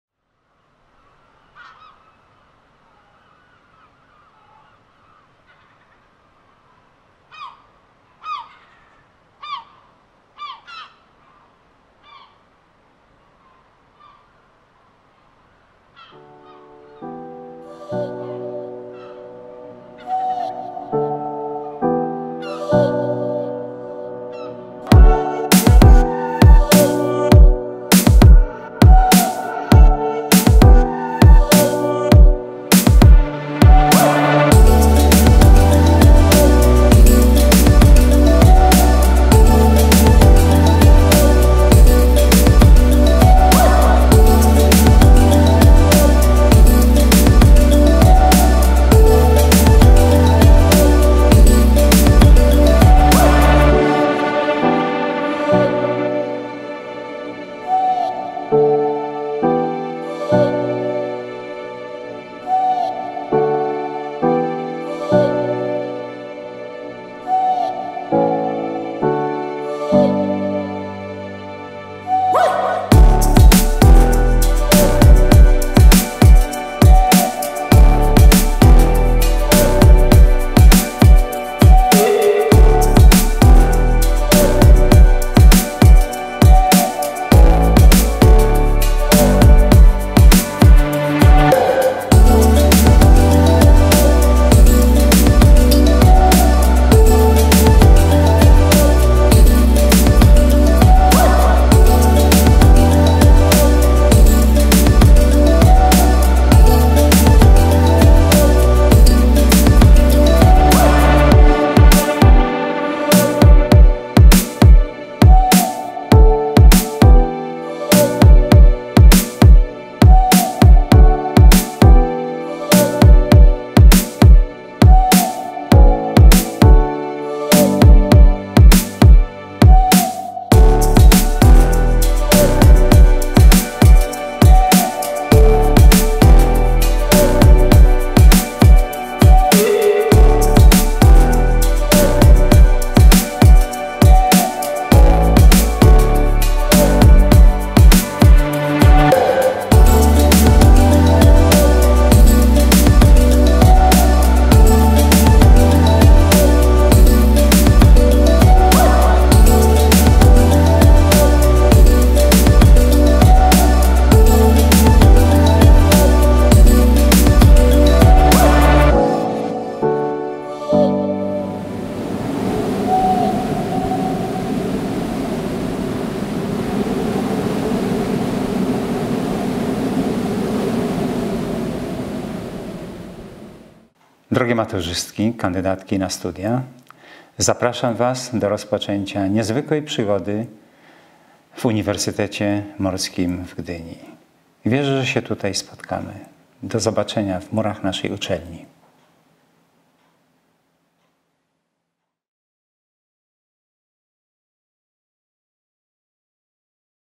Dziewczyny na Politechniki - Uniwersytet Morski w Gdyni
Film promujący kierunki techniczne Uniwersytetu Morskiego w Gdyni podczas akcji Dziewczyny na Politechniki 2020